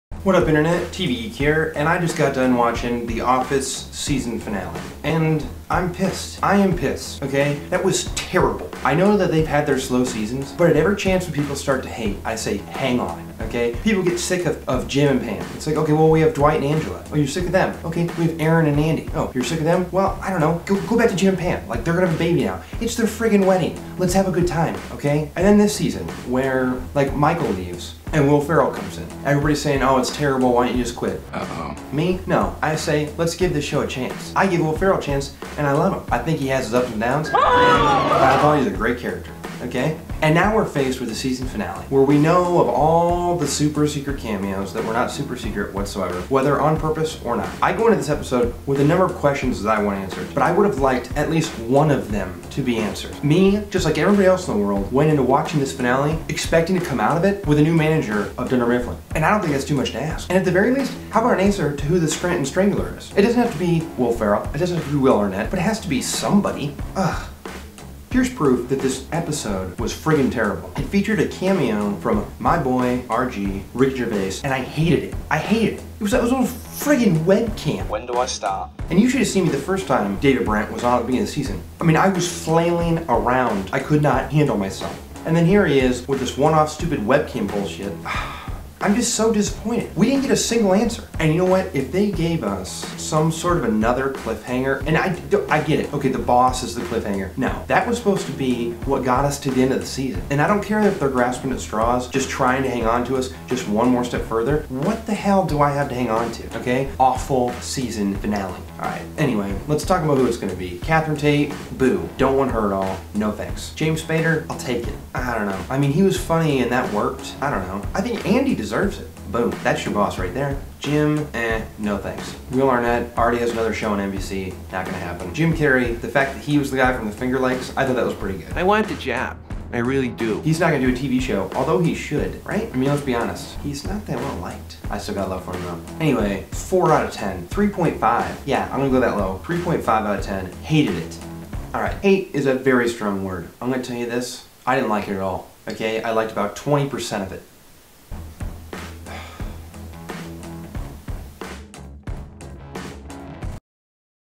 TV Geek, Episode 14, The Office Finale
Part two of my Thursday finales, sorry they're crazy late. Season two of TV Geek preview coming tomorrow.